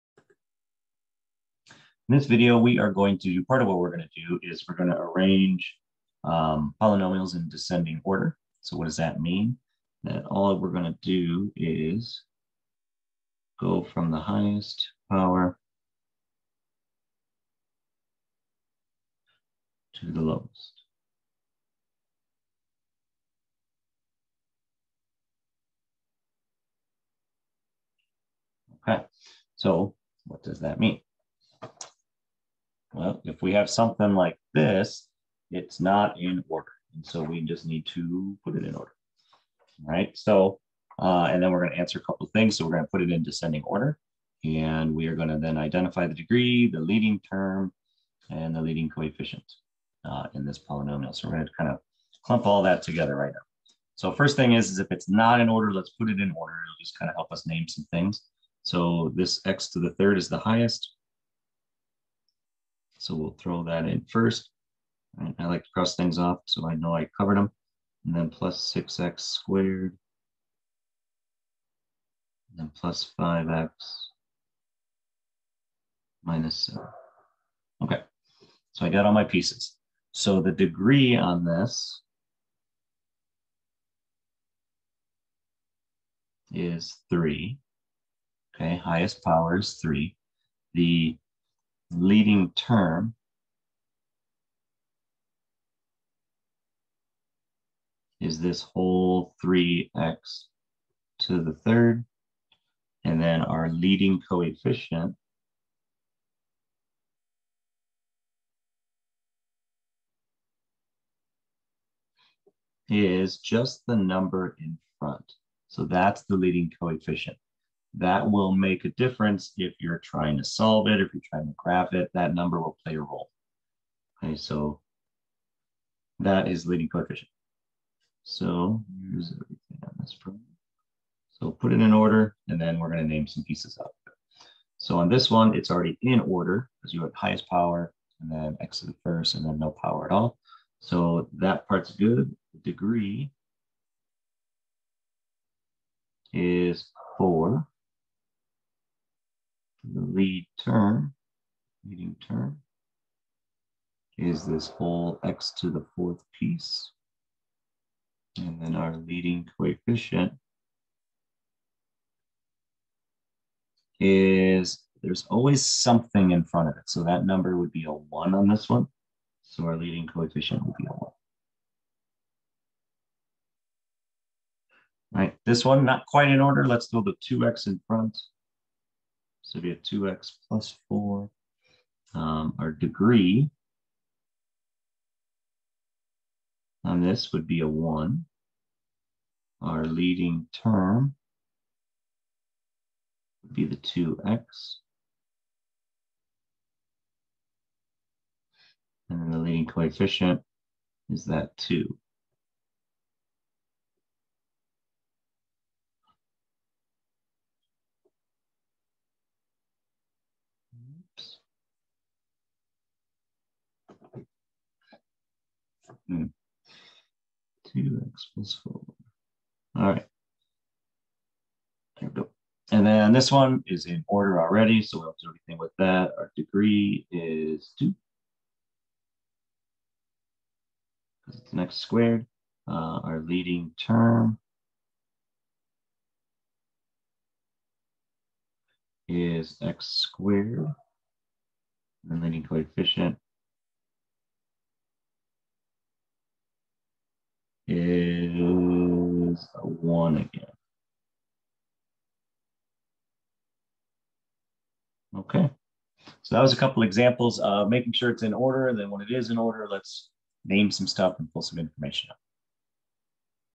Polynomials – Arranging in Descending Order
Arranging polynomials in descending order, then determining the degree, leading term, and leading coefficient.